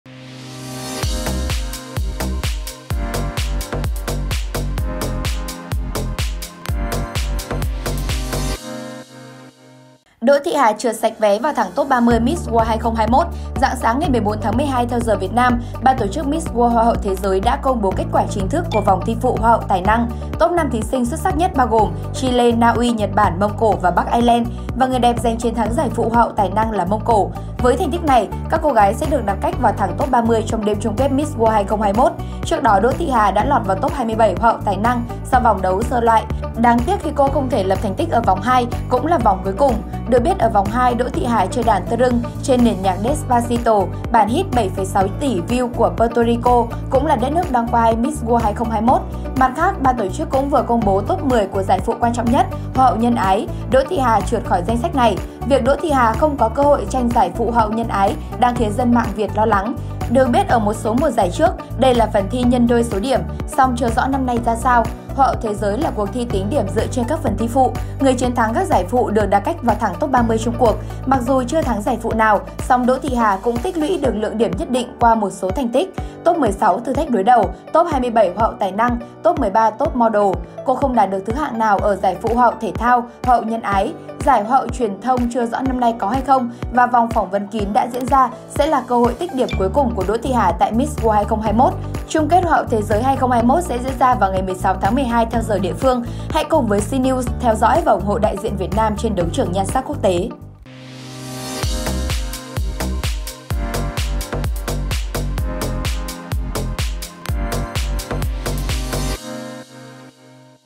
Đỗ Thị Hà TRƯỢT SẠCH các vòng thi phụ, VUỘT “TẤM VÉ” tiến thẳng vào Top 30 Miss World 2021?
Rạng sáng 14/12 (giờ Việt Nam), BTC Miss World - Hoa hậu Thế giới - công bố kết quả chính thức của vòng thi phụ Hoa hậu Tài năng. Top 5 thí sinh xuất sắc nhất gồm có: Chile, Na Uy, Nhật Bản, Mông Cổ, Bắc Ireland.

Kênh Cnews liên tục cập nhật các tin tức mới nhất trong ngày!